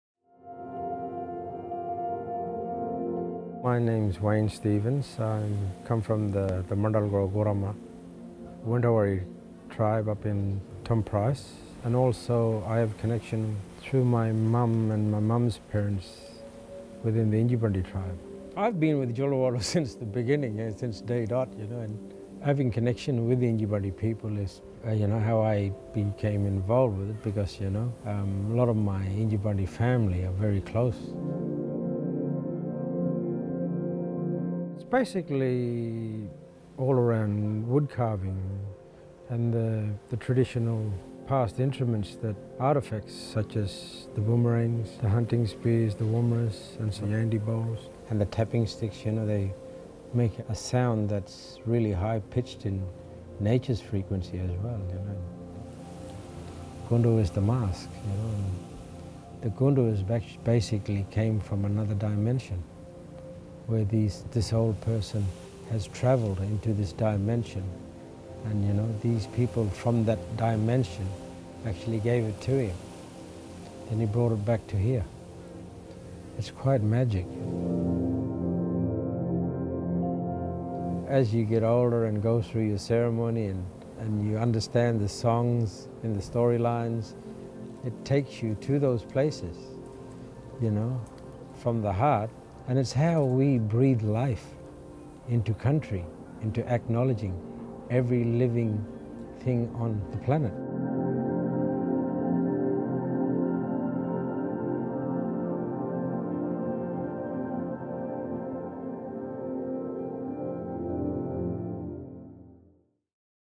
Tracks We Share: Wayne Stevens (Juluwarlu Art Group)
TRACKS WE SHARE: CONTEMPORARY ART OF THE PILBARA is a landmark exhibition celebrating the Aboriginal artists and artwork of Western Australia’s Pilbara region.

TRACKS WE SHARE: CONTEMPORARY ART OF THE PILBARA brings together more than 70 artists and over 200 artworks, and features the most exciting contemporary art coming out of the Pilbara region while paying homage to the legacy that has informed it.

A collaboration between FORM; The Art Gallery of Western Australia; Aboriginal art centres Cheeditha Art Group, Juluwarlu Art Group, Martumili Artists, Spinifex Hill Studio and Yinjaa-Barni Art; and independent artists Katie West, Curtis Taylor, and Jill Churnside; the exhibition is on display 11 March - 28 August 2022.

Video by Peacock Visuals.